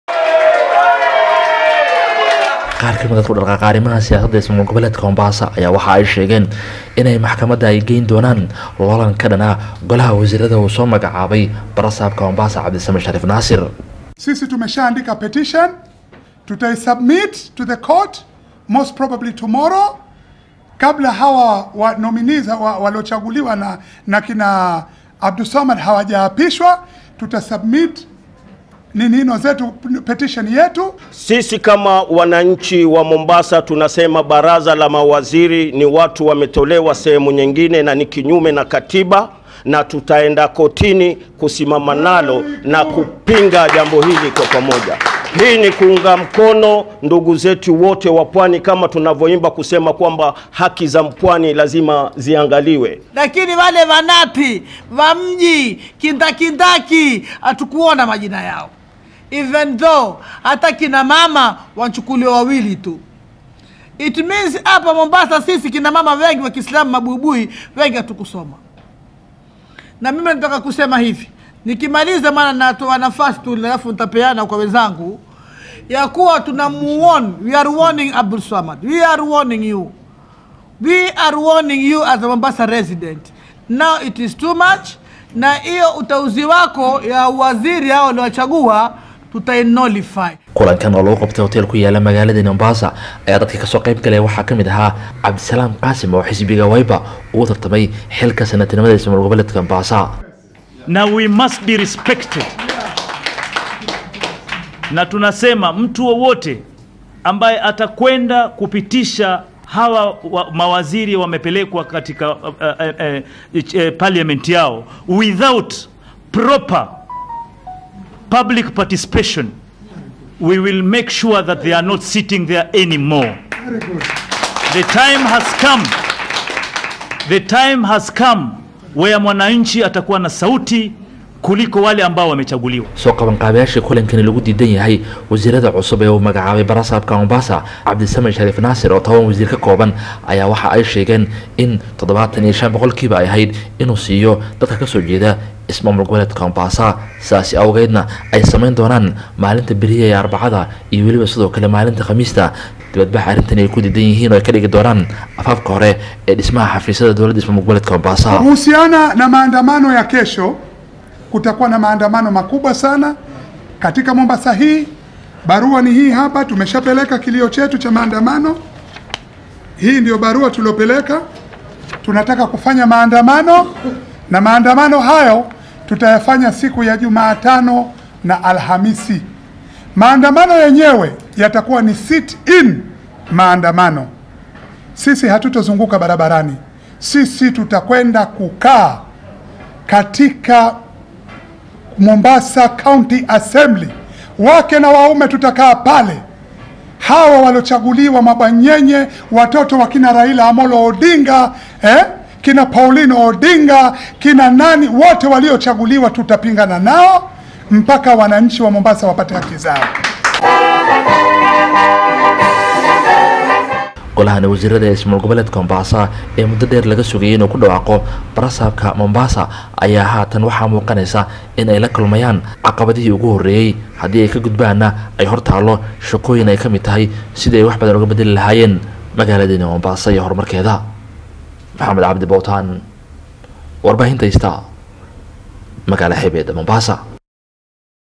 Dacwda ka dhan ah golaha wasiirrada la magacaabay ee Mombasa oo maxkamadda loo gudbinaya
Hoggaamiyayaal ka soo jeeda ismaamulka Mombasa ee gobolka Xeebta ayaa maxkamadda u gudbinaya dacwad ka dhan ah golaha wasiirrada cusub ee ismaamulkaasi ee shalay la soo magacaabay.
Hoggaamiyayaashan ayaa sidoo kale ku baaqay baannanbax maalinta berri ah.